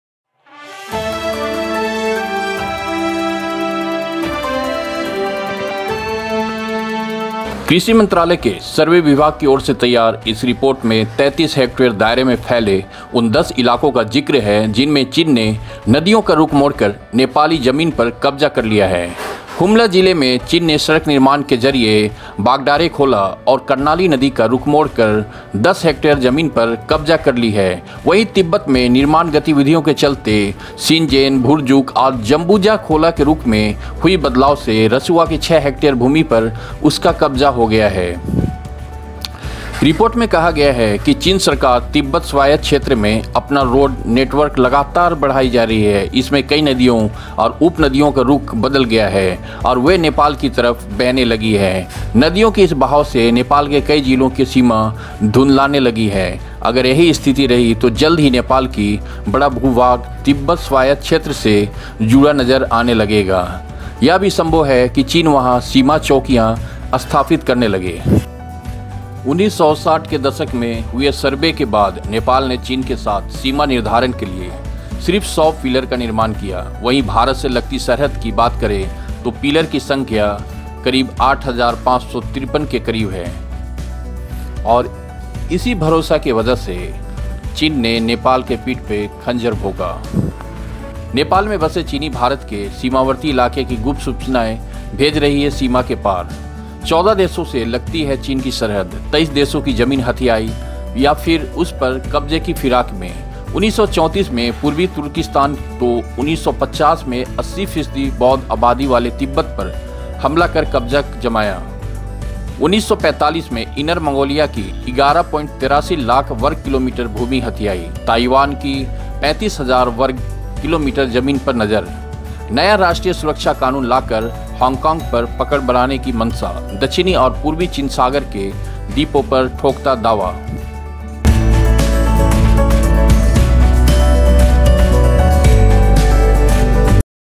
चीन ने नेपाल के10 गांव में कब्ज़ा किया
चीन ने नेपाल के10  गांव में कब्ज़ा किया चीन ने नेपाल के10  गांव में कब्ज़ा किया भारत  से नेपाल  को दुश्मनी करना महंगा पड़ा भारत के डर के वजह से  नेपाल  की ओर चीन आँख नहीं उठाता थालेकिन भारत और नेपाल के बीच बढ़ते दरार का  फायदा उठाते हुए चीन ने नेपाल को भरोसा में ले कर विश्वासघात किया शायद ये बात नेपाल को समझ में आ गया होगा,